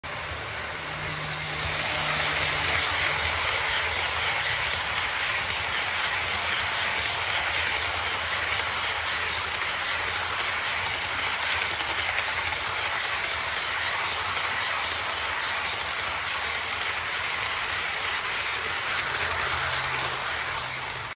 hoゲージ フラット音をお楽しみ下さい😊
何気にコンテナの積み方とかでも遊べる^_^、なかなかかったるくてやらないですが　全部おろして　実写を見たりして積み直す遊び方^_^